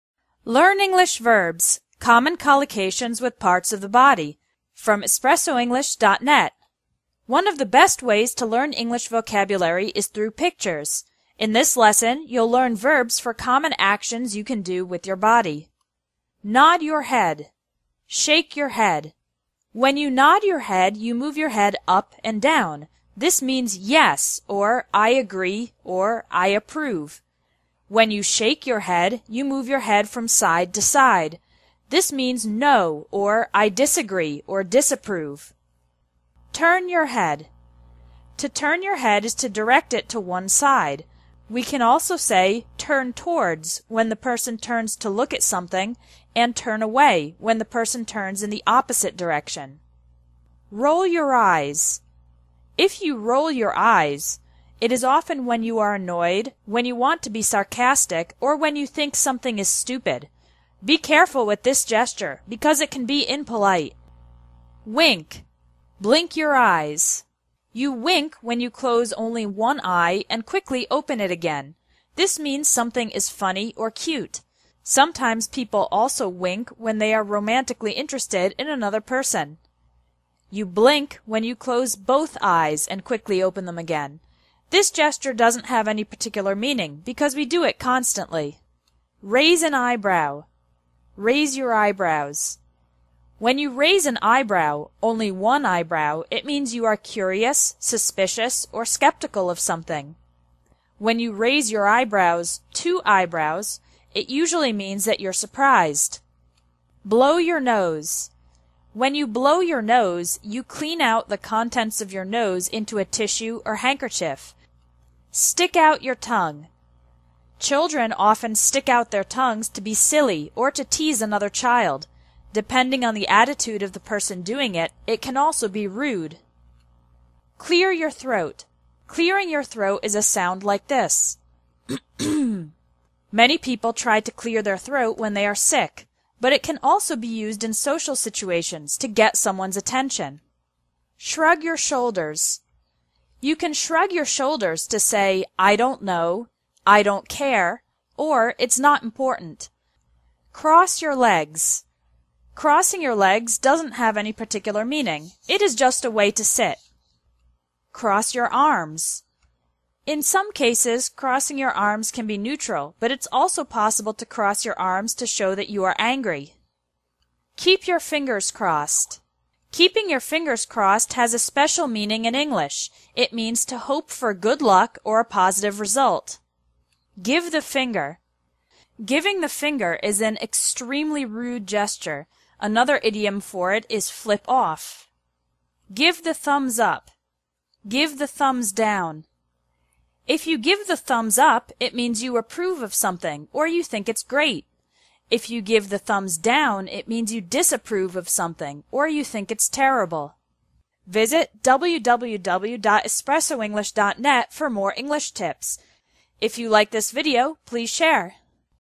Verbs in English: Expressions with Parts of the Body
Learn common English expressions and phrases with parts of the body. for more English tips!